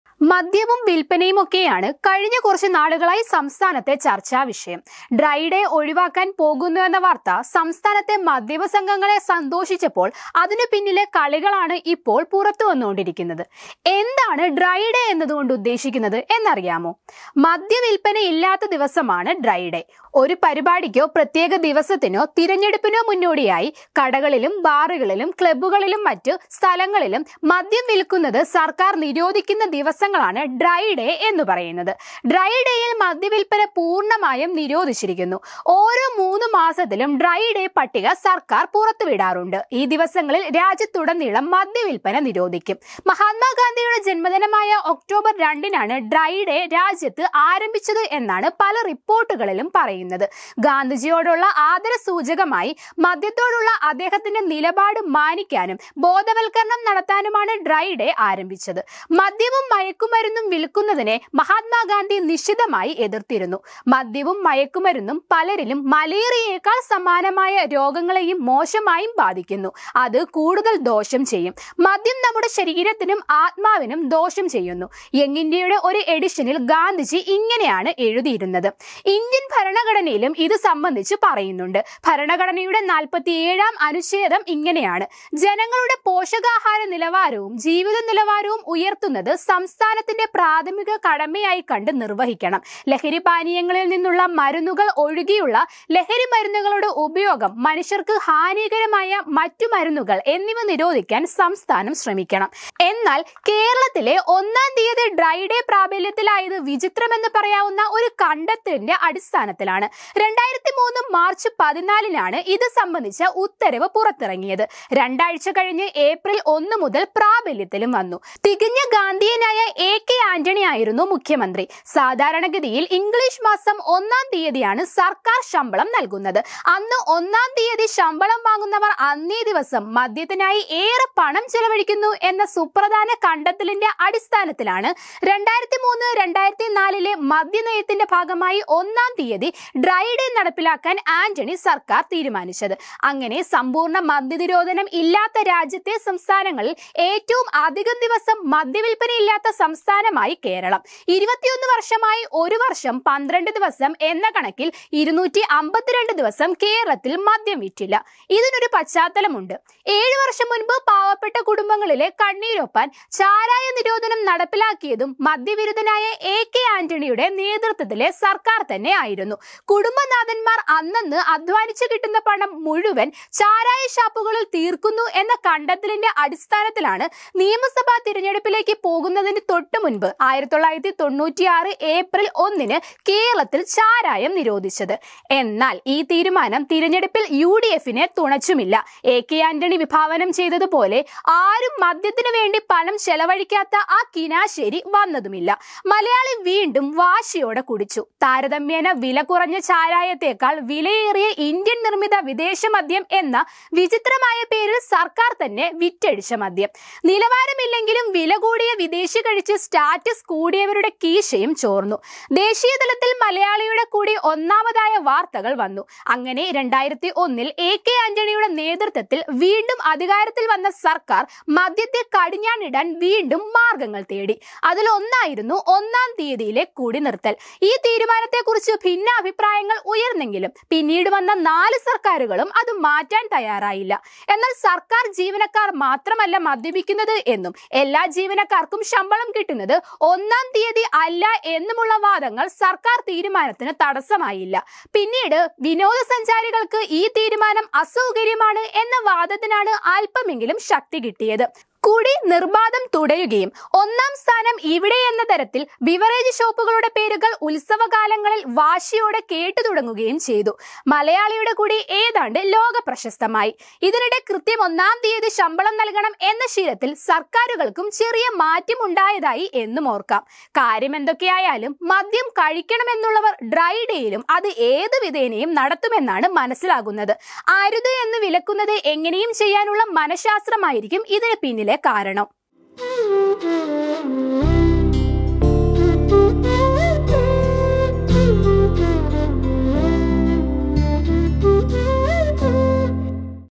ഡ്രൈ ഡേയും ശമ്പള ദിനവുമായി എന്താണ് ബന്ധം ? | Kerala | Dry day
Many reports say that Dry Day started in the country on October 2, Mahatma Gandhi's birthday. Dry Day was started as a tribute to Gandhi to respect his stance on alcohol and raise awareness. Mahatma Gandhi strongly opposed the sale of liquor and drugs. | മഹാത്മാഗാന്ധിയുടെ ജന്മദിനമായ ഒക്ടോബർ 2 നാണ് ഡ്രൈ ഡേ രാജ്യത്ത് ആരംഭിച്ചതെന്നാണ് പല റിപ്പോർട്ടുകളിലും പറയുന്നത്. ഗാന്ധിജിയോടുള്ള ആദരസൂചകമായി മദ്യത്തോടുള്ള അദ്ദേഹത്തിന്റെ നിലപാട് മാനിക്കാനും ബോധവൽക്കരണം നടത്താനുമാണ് ഡ്രൈ ഡേ ആരംഭിച്ചത്. മദ്യവും മയക്കുമരുന്നും വിൽക്കുന്നതിനെ മഹാത്മാഗാന്ധി നിശിതമായി എതിർത്തു.

"നാടിനൊപ്പം നേരിനൊപ്പം"